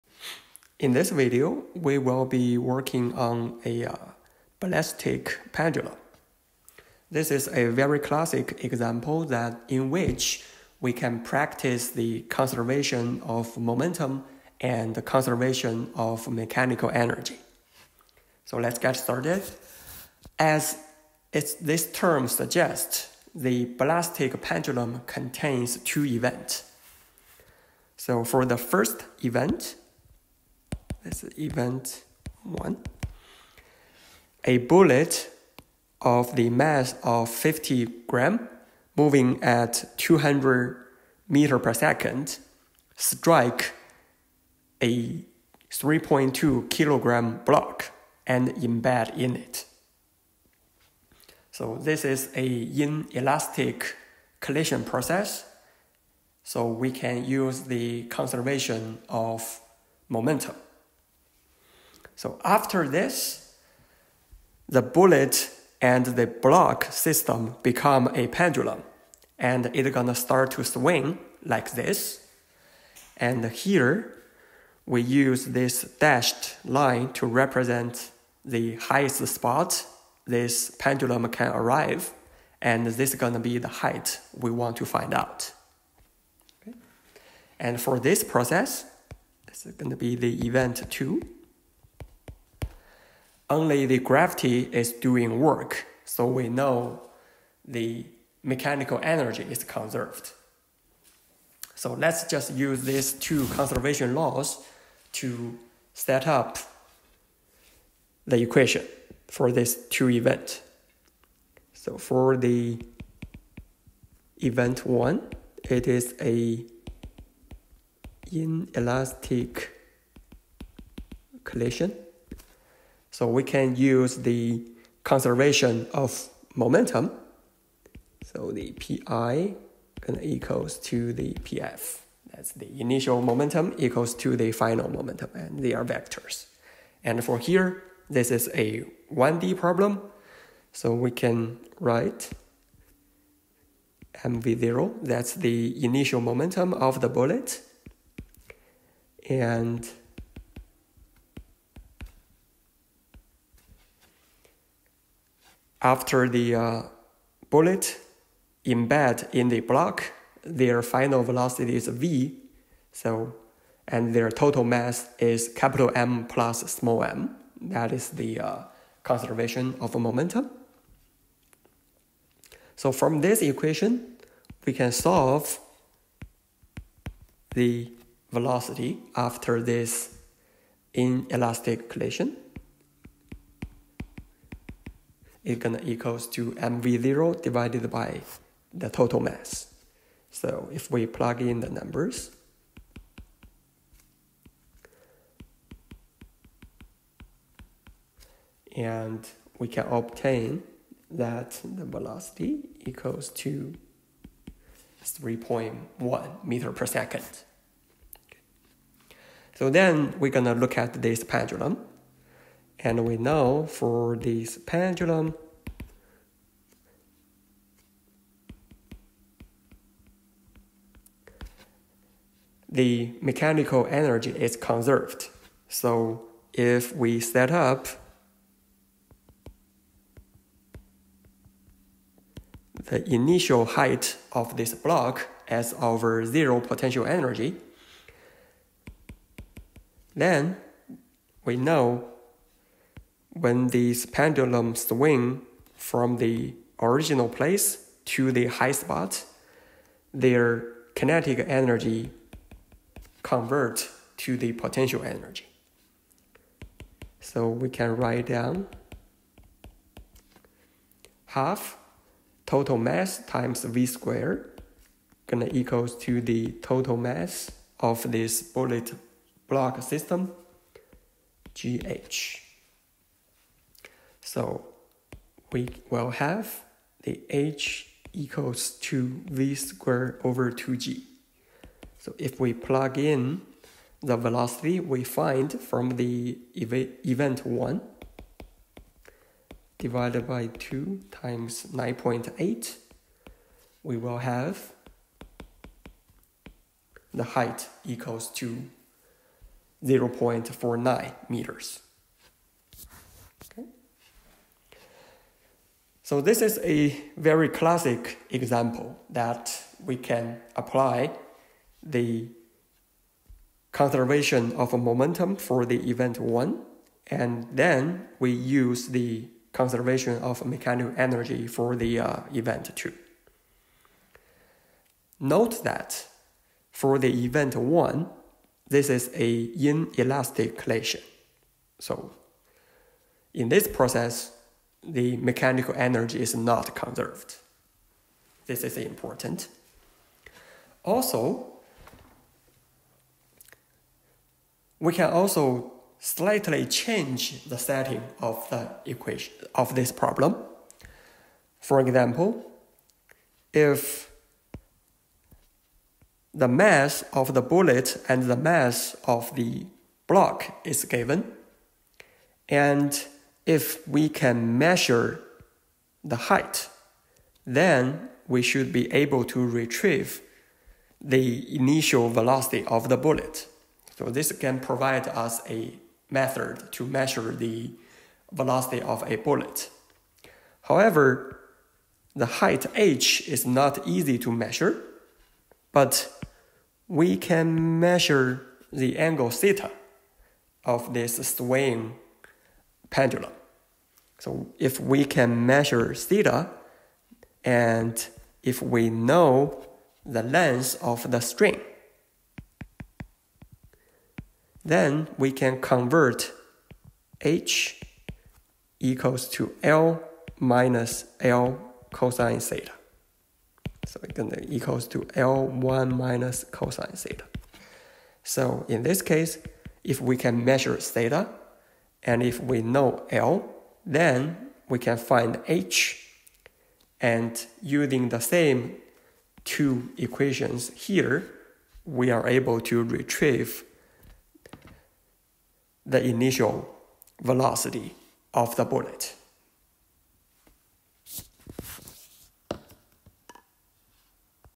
Ballistic pendulum
This video shows how to solve a ballistic pendulum problem. First, a bullet strikes a hanging block and embeds in it. This is an inelastic collision, in which the momentum is conserved while the mechanical energy is not. Then the bullet-block system swings to a height of h. Using conservation of mechanical energy, we can find h.